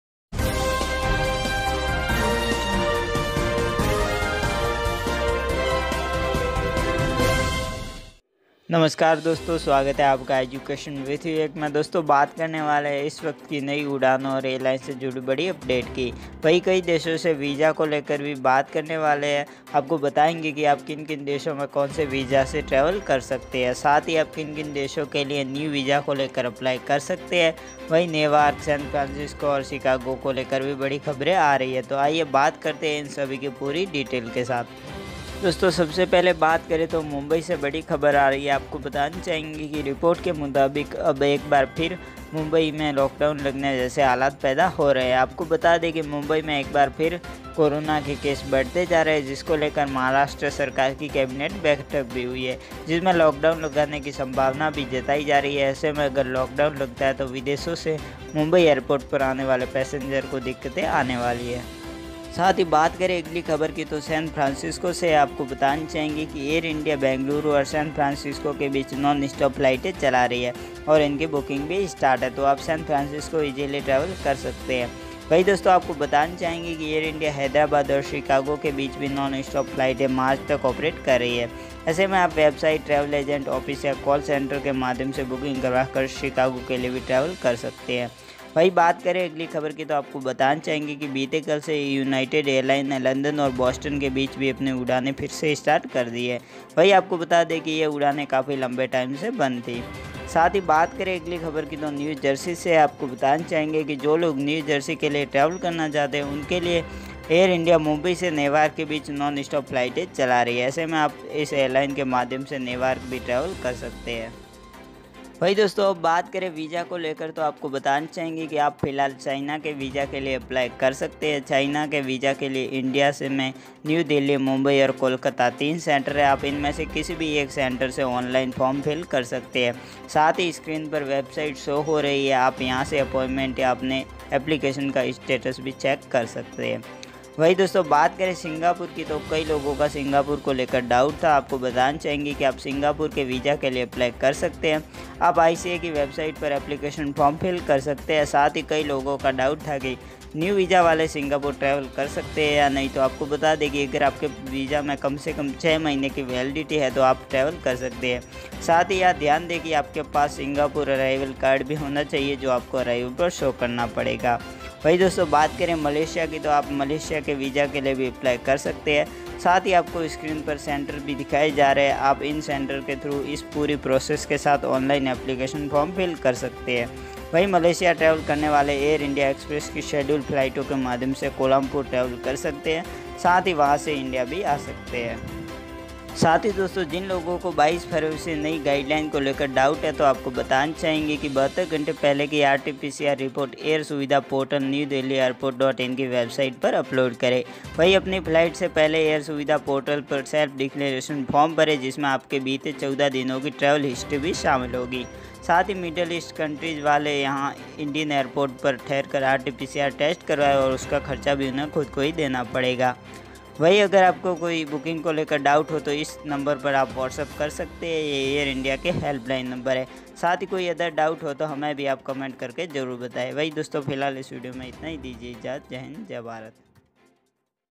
International flight latest update ||Malaysia,Singapore,And VISA related big update
SHOW LESS